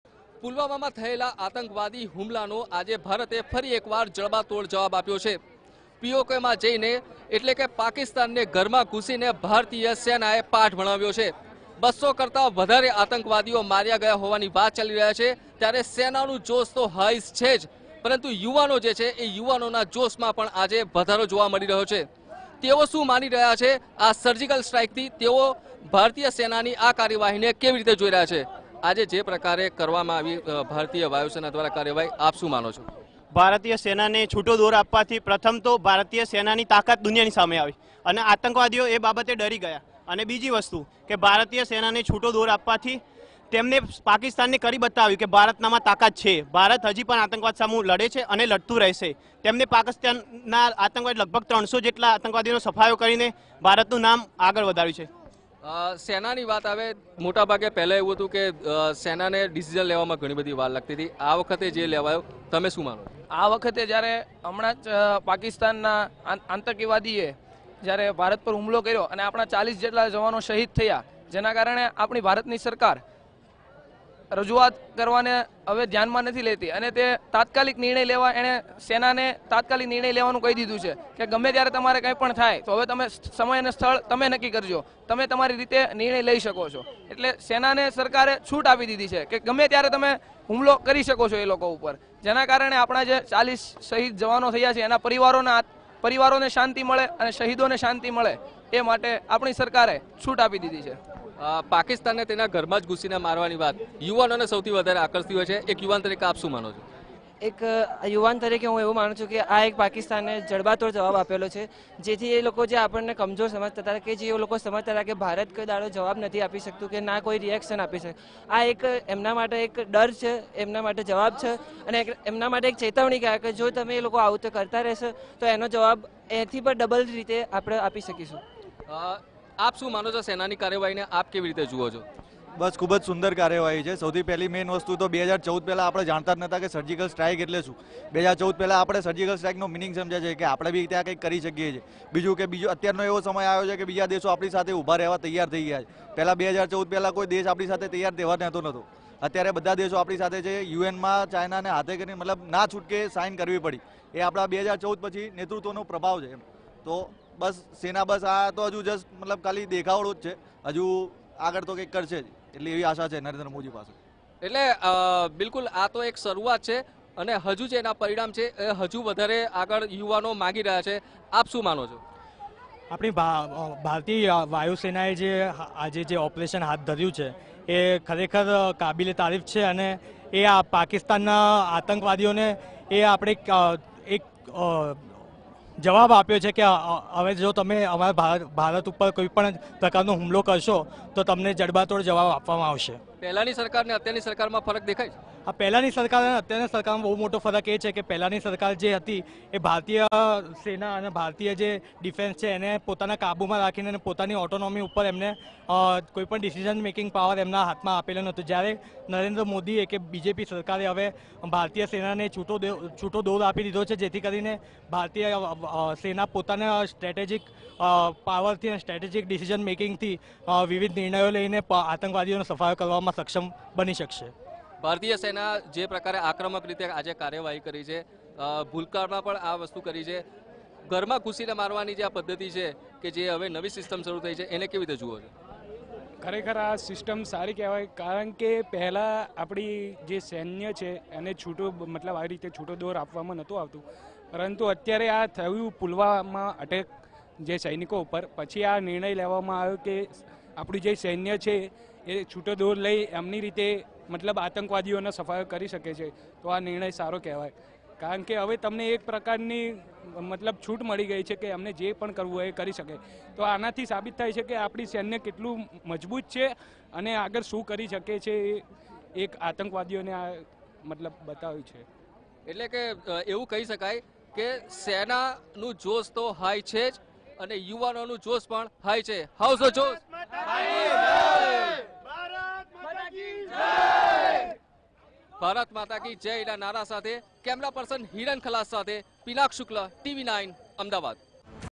Ahmedabad: People's reaction after IAF carried out aerial strikes at major terror camps in PoK- Tv9
Ahmedabad: People's reaction after IAF carried out aerial strikes at major terror camps in PoK